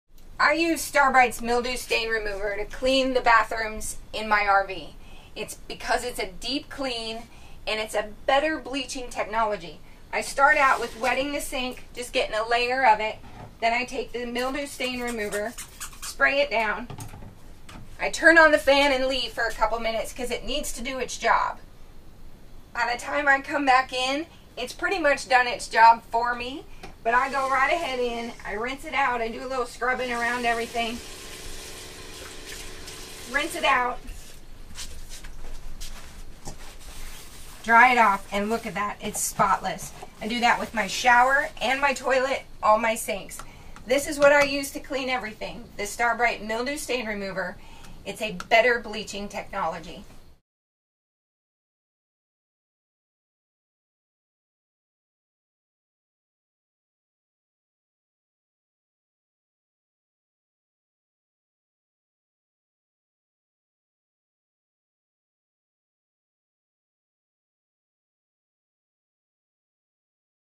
💦 RV Bathroom Cleaning Solution - Star Brite Mildew Stain Remover
Looking for an easy RV kitchen bathroom toilet cleaning solution? Patrice shows us what to use for deep cleaning the RV bathroom toilet and kitchen sink with Star Brite Mildew Stain Remover - a better bleach technology.

💦 Mildew Stain Remover

▬▬▬▬ More RV There Yet ▬▬▬▬